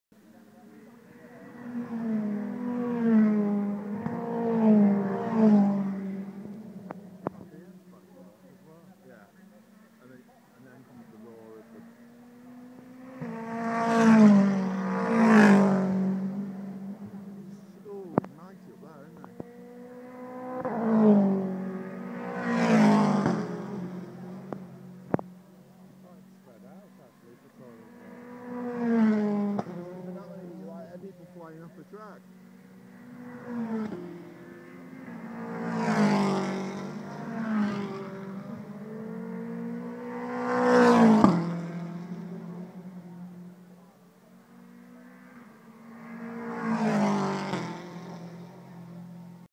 BTCC Oulton Park 2012
Clip from race two of the touring cars